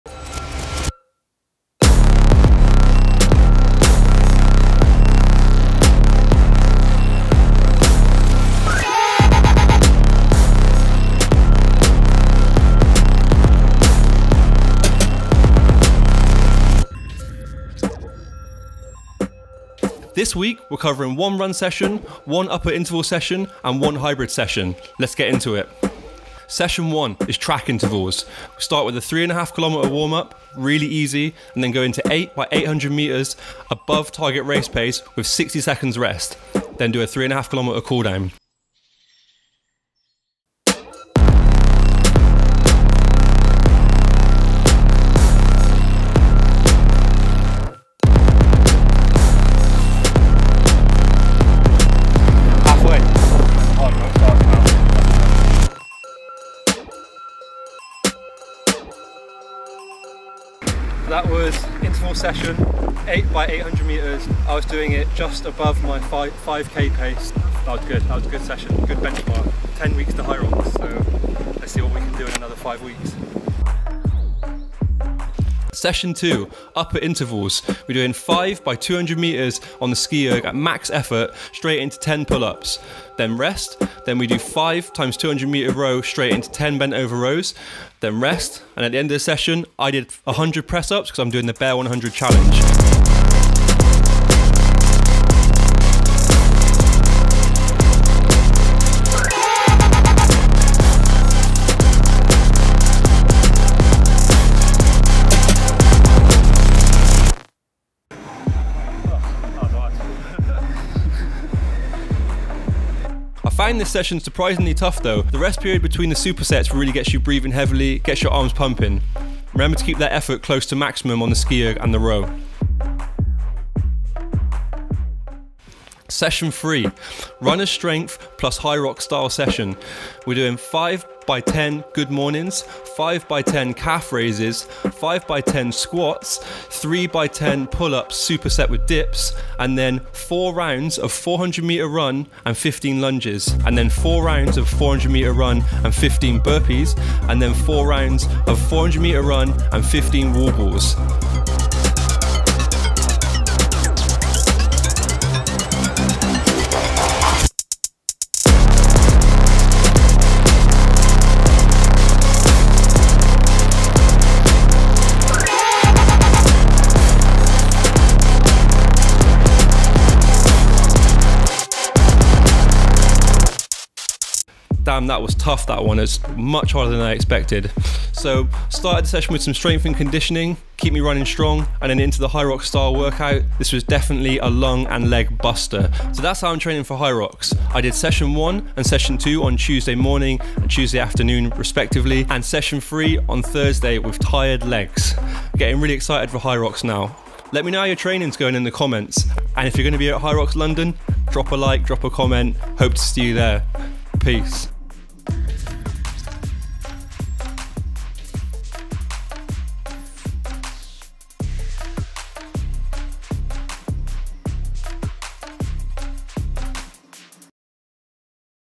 How I'm training for HYROX 02 | Interval training
An insight into the types of sessions I'm doing to mentally and physically prepare myself for Hyrox

These sessions are aimed at increasing V02 max and muscular endurance. Between these sessions I do zone 2 cardio, it's not glamorous but it gets the job done. These kind of exercises are useless without building a strong aerobic base.

Session breakdown -

Session 01
Track intervals
3.5km warm up
8 x 800m (above target race pace) 60s rest
3.5km cool down

Session 02
Upper intervals
5 x (200m ski erg straight into 10 pull ups)
rest
5 x (200 row straight into 10 bent over rows)
rest
100 press ups

Session 03
Runners strength + HYROX session
5 x 10 Good mornings
5 x 10 Calf raises
5 x 10 Squats
3 x 10 pull ups / dips
4 x (400m run / 15 lunges)
4 x (400m run / 15 burpees)
4 x (400m run / 15 wall balls)